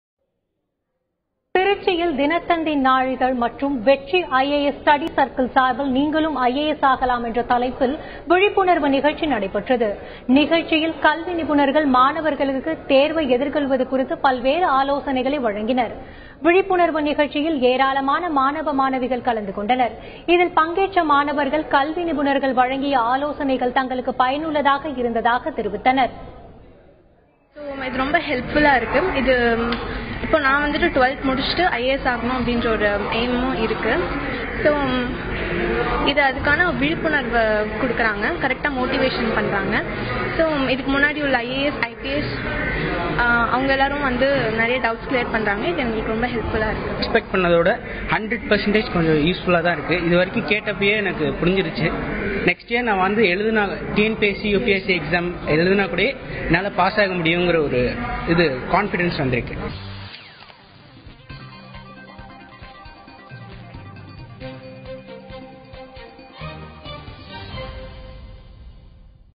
Neengalum aagalam IAS |Trichy | Vetrii IAS Study Circle
Vetrii IAS academy in association with Dhinathanthi provides free seminar ""Neengalum aagalam IAS"" at Trichy on Sunday 26th May 2019.